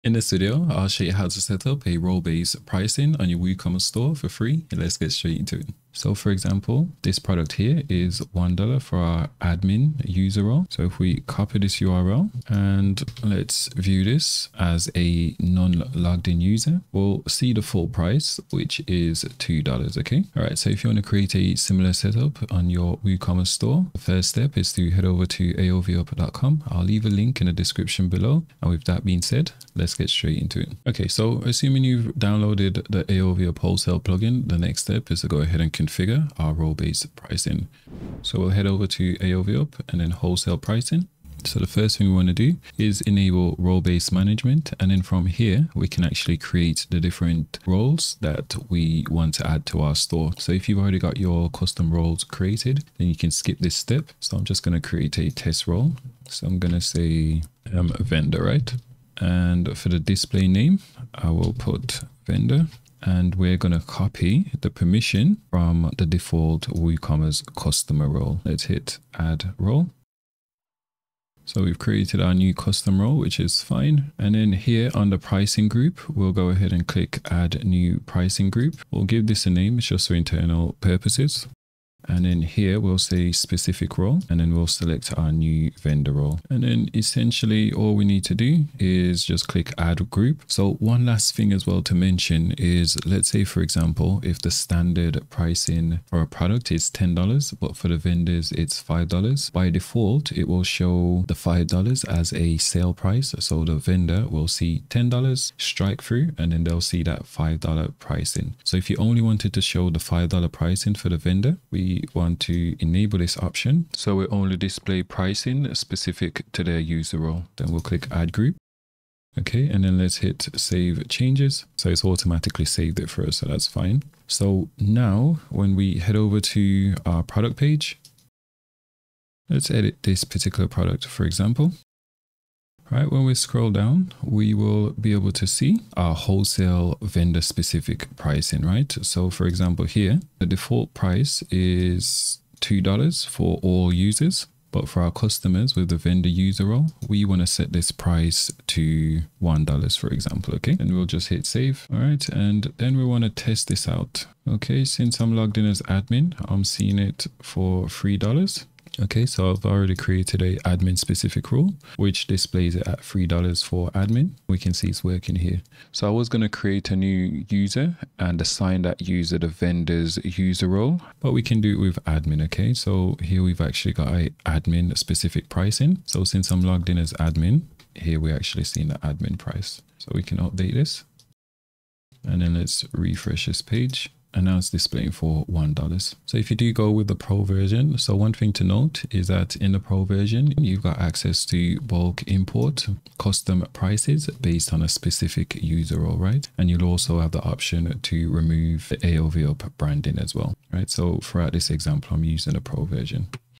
Free Role Based Price for WooCommerce (Easy setup)
I'll show you how to create a role-based price for WooCommerce.

00:00 Intro
00:05 Overview
00:20 Where to download the plugin
00:30 Installation
00:41 Create custom user roles
01:14 Create pricing group
02:09 Add role-based pricing
03:02 Test